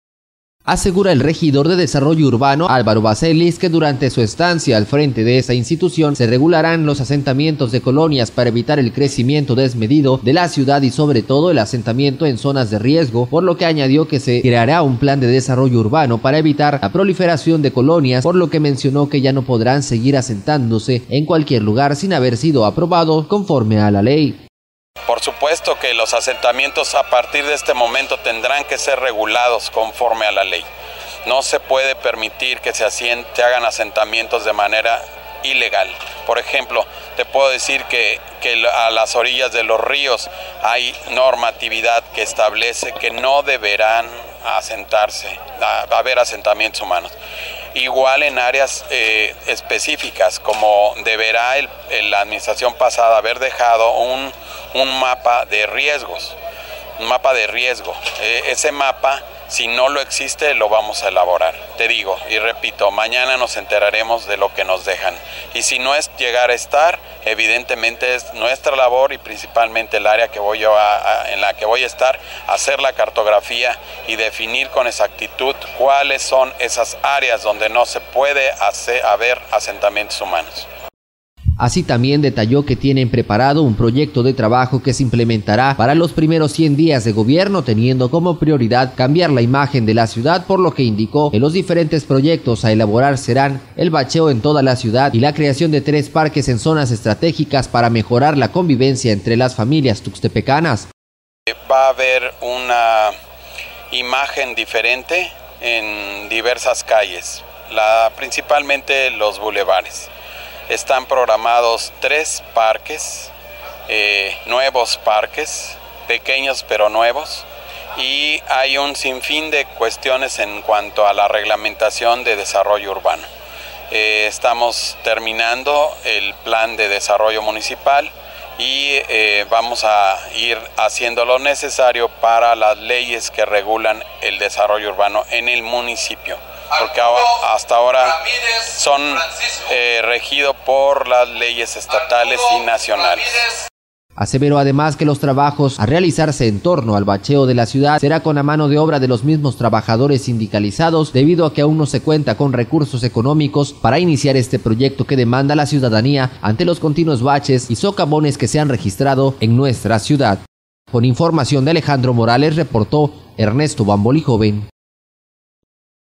Necesario regular asentamientos de colonias: Desarrollo Urbano
-Señala Álvaro Bacelis que la prioridad en los 100 días de Gobierno de Dávila será cambiar la imagen de Tuxtepec.